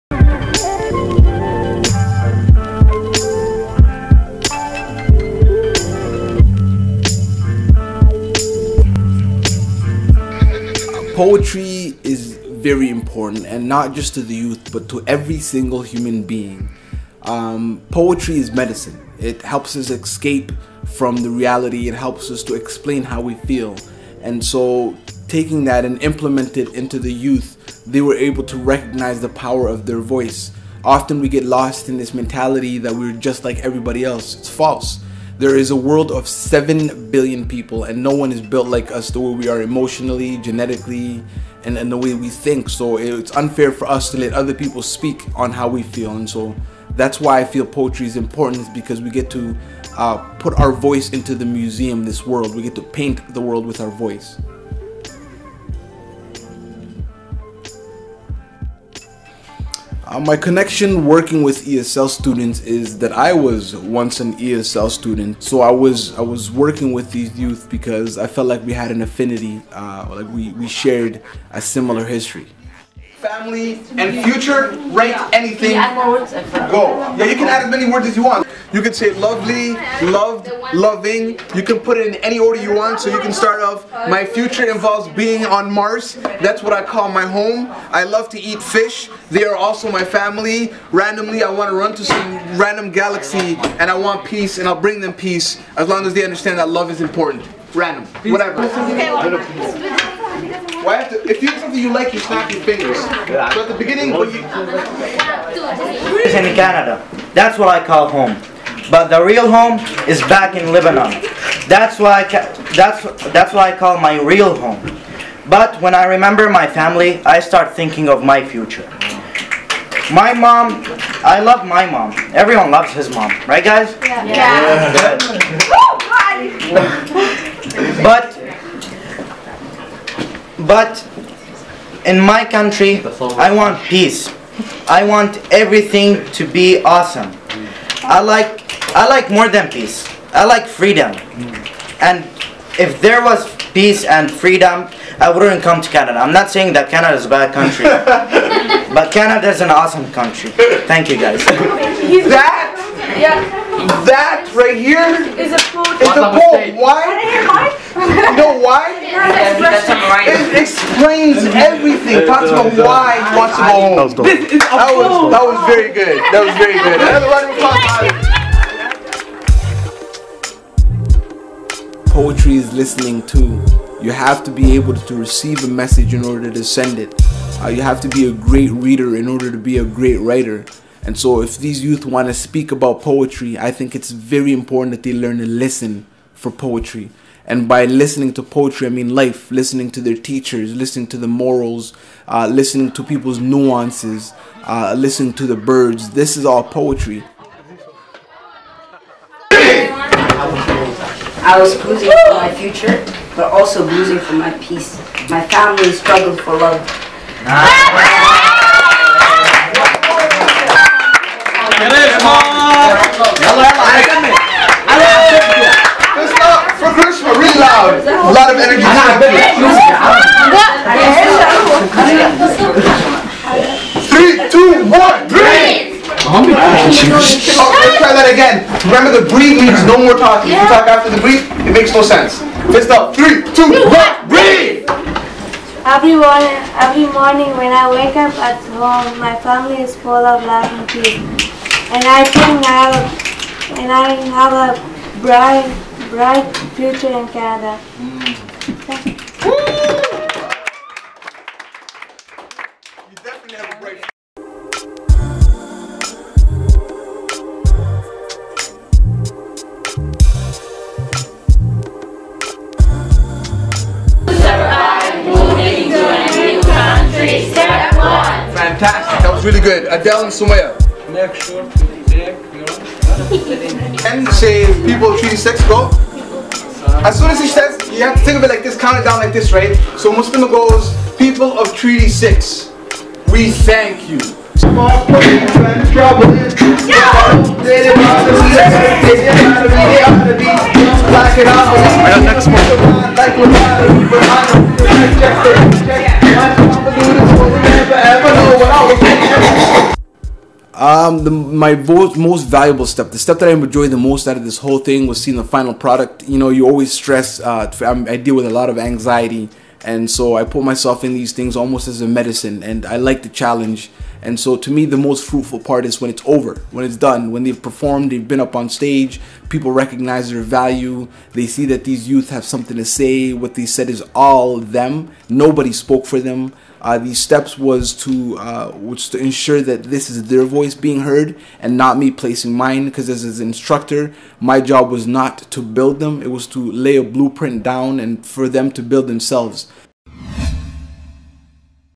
Integration TV: Poetic Beginnings: Teaching & Inspiring Youth!
We visit Edmonton, Alberta and meet an inspiring Poet/Performer Ahmed Knowmadic who is using his craft to inspire young newcomers to Canada. Using his ancestors tradition of Poetry, Ahmed has become a trailblazer in his hometown!